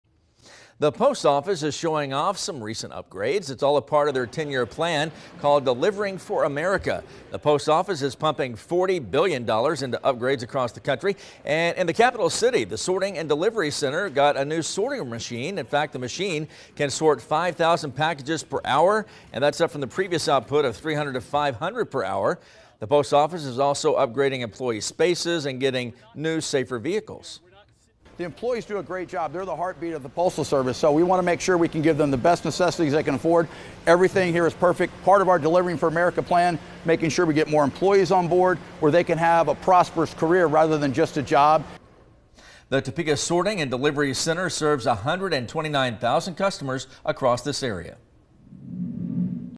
$40 billion national investment makes it’s way to Topeka
A federal agency’s $40 billion investment is bringing major upgrades for postal workers at Topeka’s Sorting and Delivery Center.